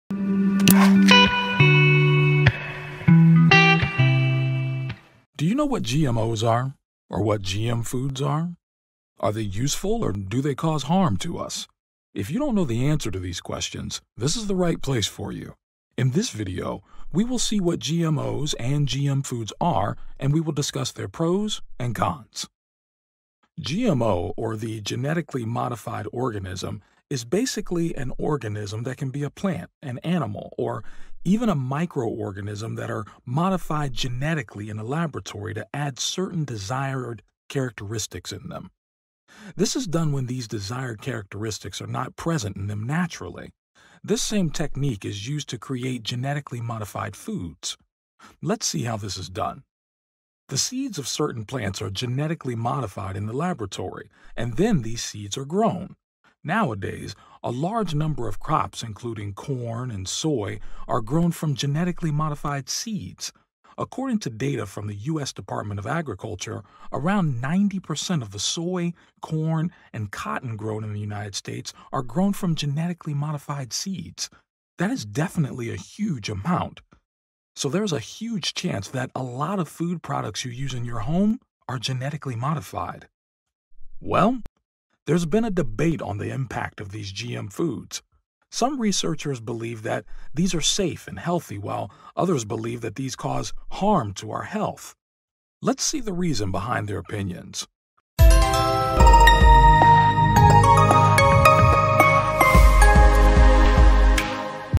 Genetically modified foods effects on human health - environmental science
Genetically modified foods effect on human health.

👀 Check out the accompanying blog post on Dynamic Earth Learning's

🖳 See Dynamic Earth Learning's online environmental science courses!